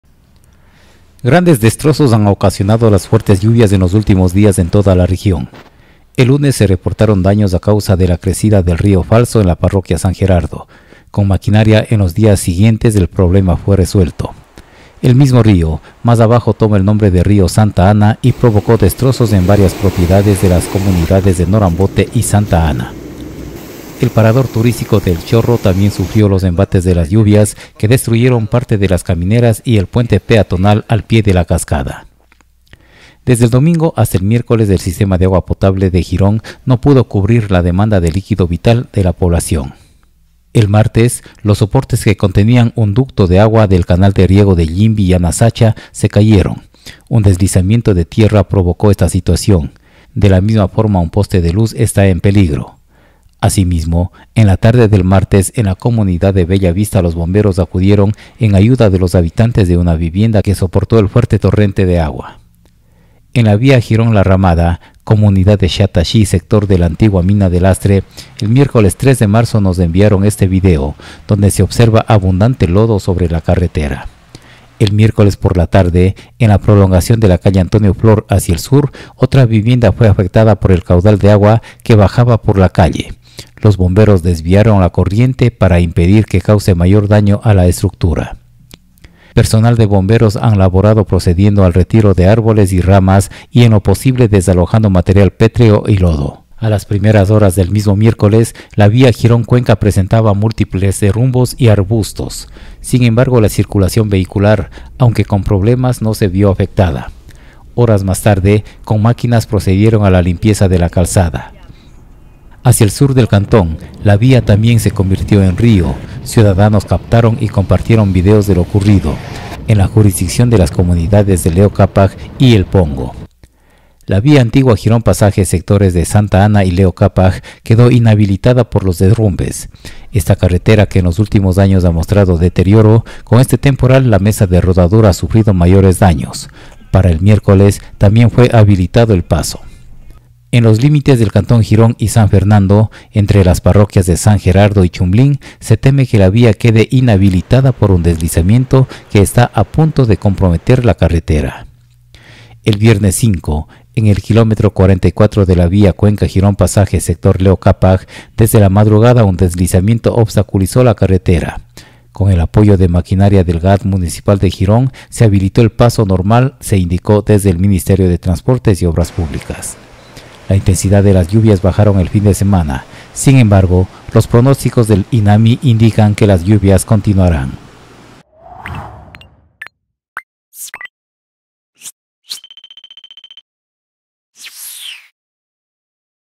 Seis días de destrozos en las vías y otros lugares provocaron las intensas lluvias
Desde el 28 de febrero hasta el 5 de marzo, el fuerte temporal invernal provocó múltiples afecciones a la vialidad, viviendas, estructuras, en la jurisdicción del cantón Girón. Algunos de los daños fueron atendidos por las instituciones respectivas, lo cual ha hecho que no se suspenda el tráfico vehicular; dos viviendas fueron afectadas por la correntada; y, un paso a desnivel que soporta un canal de riego se encuentra inhabilitado.